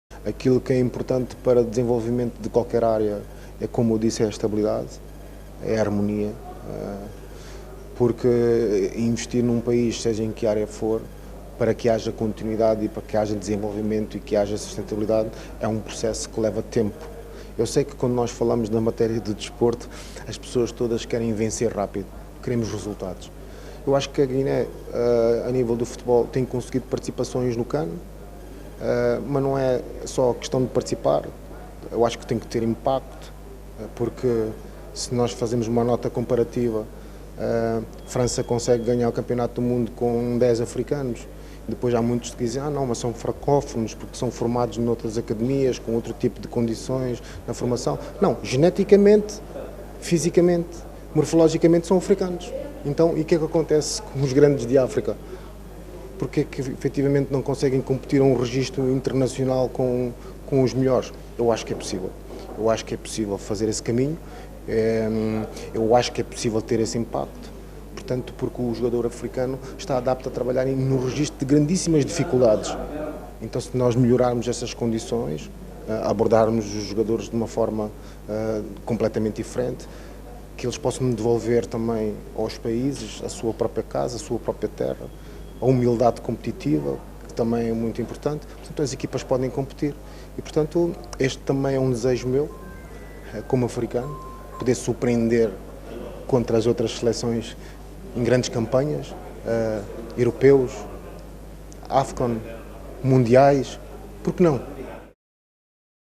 Abel Xavier apela para paz e estabilidade da Guiné-Bissau
O antigo futebolista luso-moçambicano Abel Xavier apelou hoje para a necessidade de haver paz e estabilidade na Guiné-Bissau, para que o país possa enfrentar os desafios do futuro e potenciar o talento da juventude.